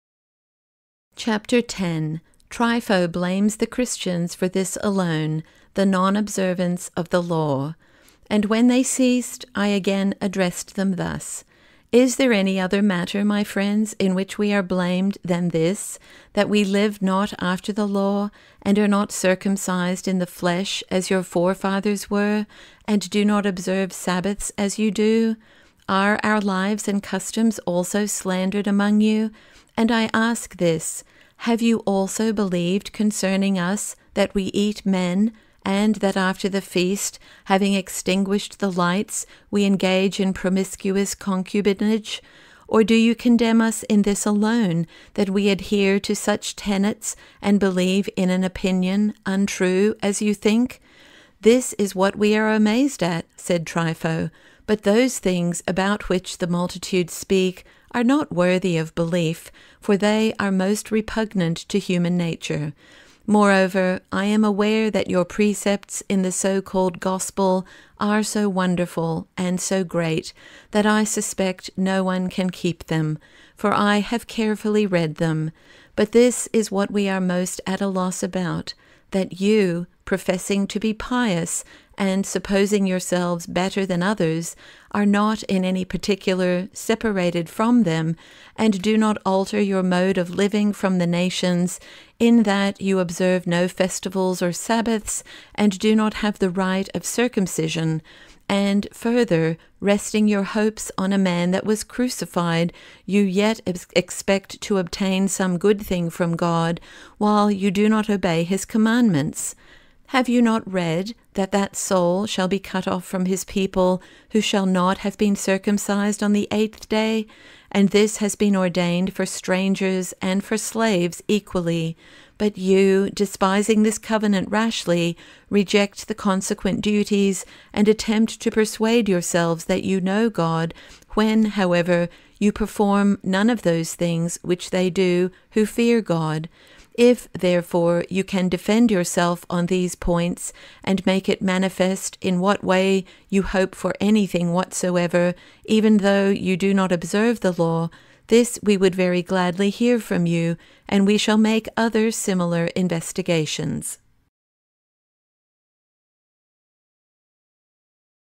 Chapter X - Dialogue of Justin Martyr with Trypho (Justin Martyr) audiobook
Justin Martyr (c. AD 100-165) was an early Christian apologist and philosopher. Most of his works are lost, but two apologies, a dialogue, and a few fragments have survived. A student of several schools of philosophy, notably Platonism, he found none of them satisfactory. An old Syrian Christian challenged him in a dialogue, explaining how the testimony of the prophets are more reliable than the reasoning of philosophers. Moved by the aged man's argument, Justin renounced both his former religious faith and his philosophical background, choosing instead to re-dedicate his life to the service of the Divine. His conversion is generally thought to have occurred at Ephesus. He traveled throughout the land, spreading the knowledge of Christianity as the "true philosophy," starting his own school in Rome. Because of his refusal to sacrifice to Roman idols, Justin was martyred, along with six of his friends and students.

The Dialogue with Trypho, along with the First and Second Apologies, is a second-century Christian apologetic text, usually agreed to be dated in between AD 155-160. It is seen as documenting the attempts by theologian Justin Martyr to show that Christianity is the new law for all men, and to prove from Scripture that Jesus is the Messiah.